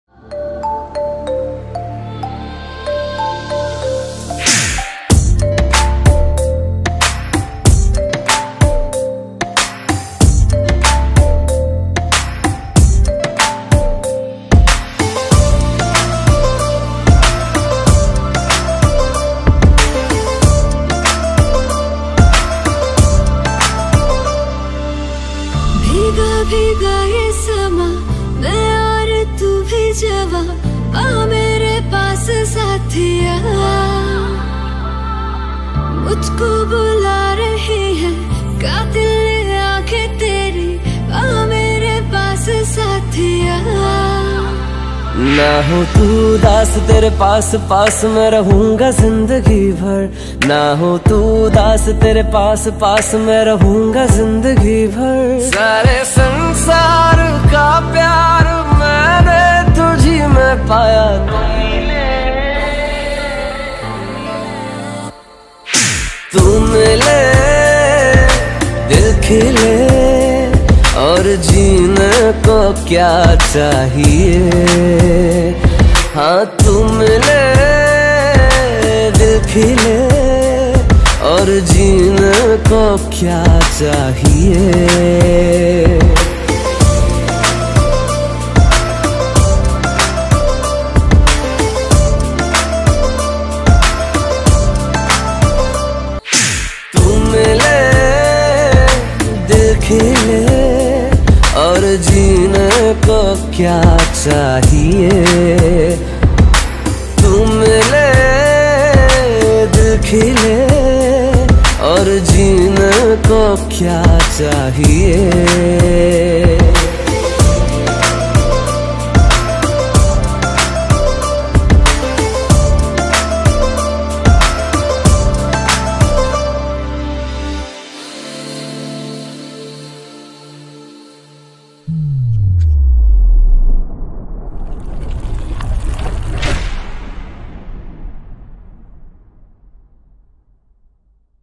Holud dance perfomens | Nayema | Holud Night || Mirror Focus
The HOLUD performance you should NOT miss!! The dramatic love story of the Bride & Groom is enacted phenomenally by the TEAM Bride & Groom performances.

Get your mind boggled and your heart thrilled watching the 'OSTHIR' performance!